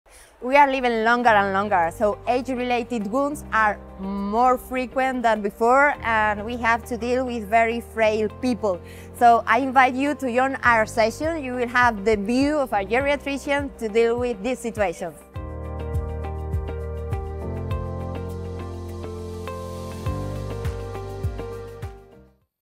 Join our geriatric session at EWMA 2025
Join us at the EWMA-GNEAUPP 2025 Conference in Barcelona, where we will present a session focusing on age-related wounds!

We hope to see you in Barcelona! ✨